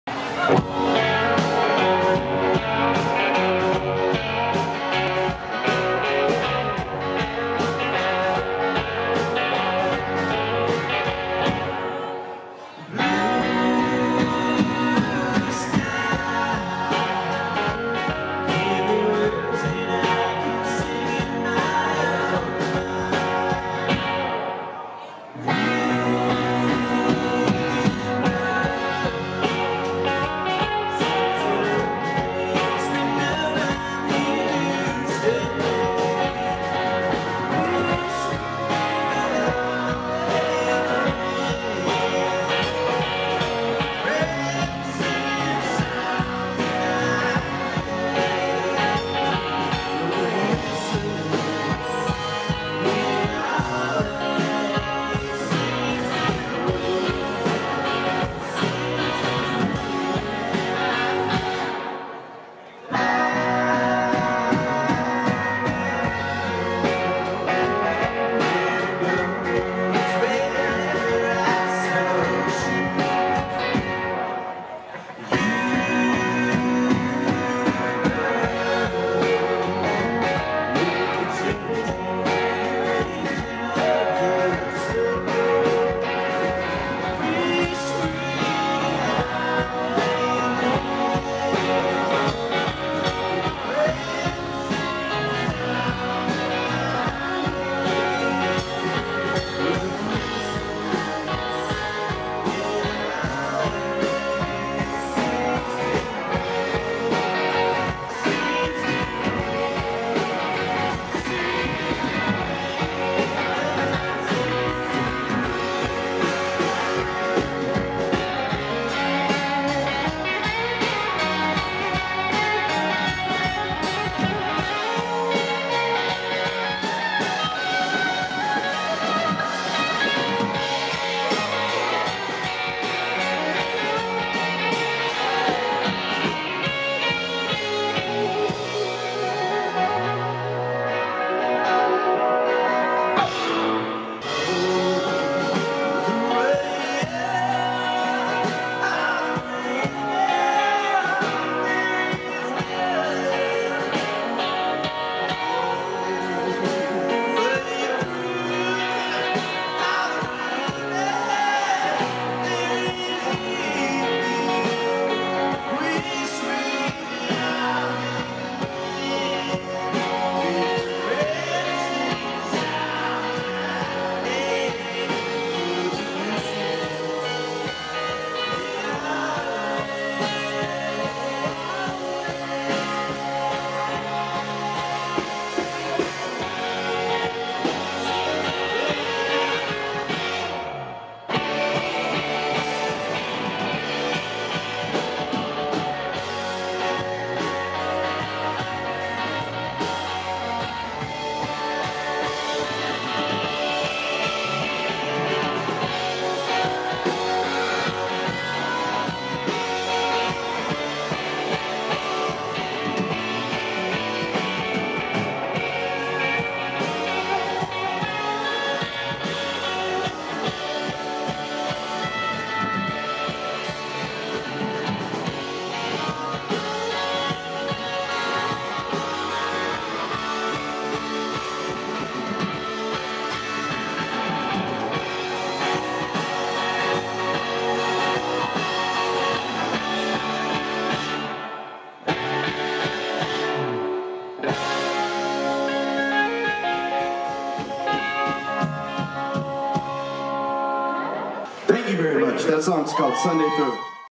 +Rusty Dusty, Wild_Austin_music!
This band- headliner, the other night., at the Parish, in downtown Austin.  So cool!